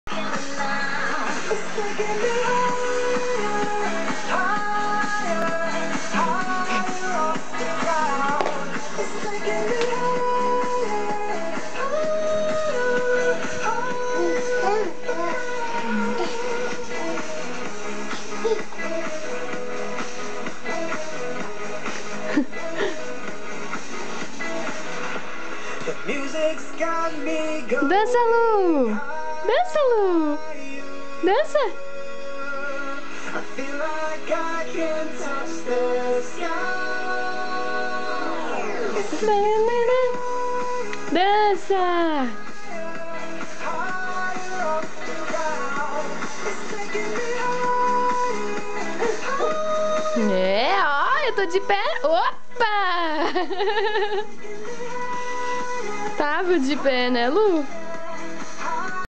Garota tecnologia!
Luiza agora já fica de pé soziiiinha!!! Adora uma  boa música para dançar, não larga o celular e fica ligada na tv sempre que possível! Hahahaha
Reparem que ela está sem se apoiar em nada... Até tenta dançar, mas não dá mto certo ainda... Liiinda!!

Rio Negrinho, Maio/2011